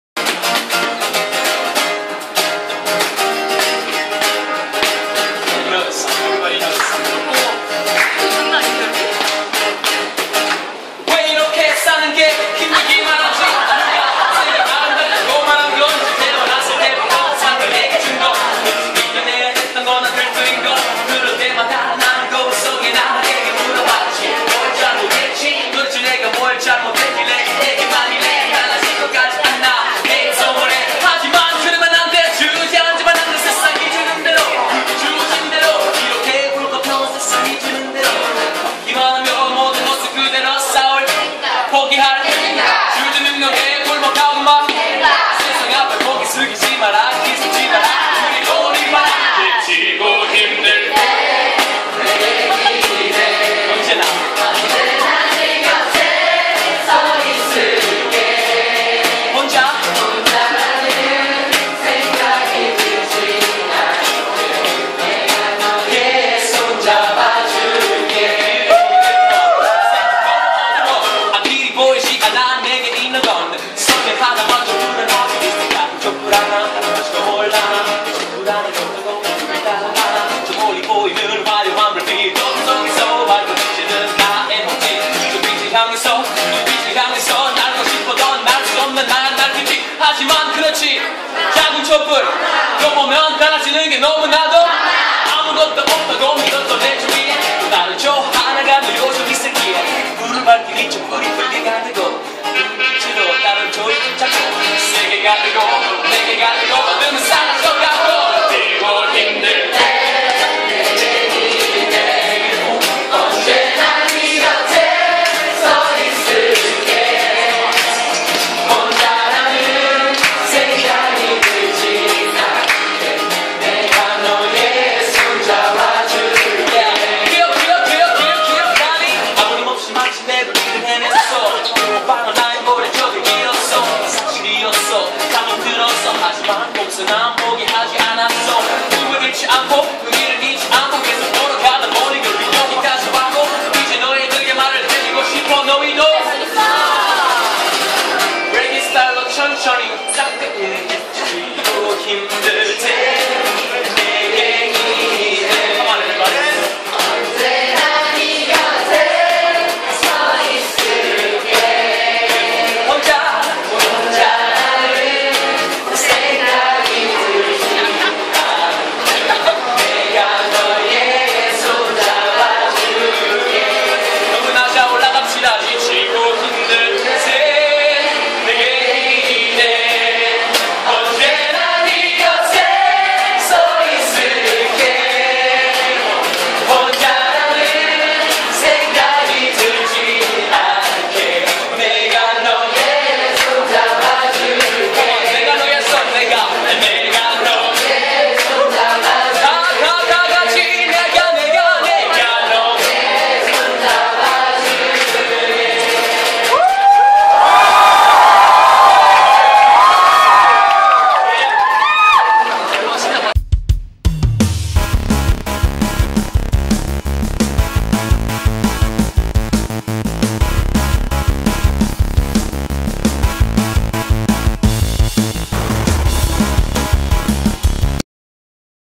People in South Korean Subway Join in on a Song
This man singing in a South Korean Subway gets joined by people around him.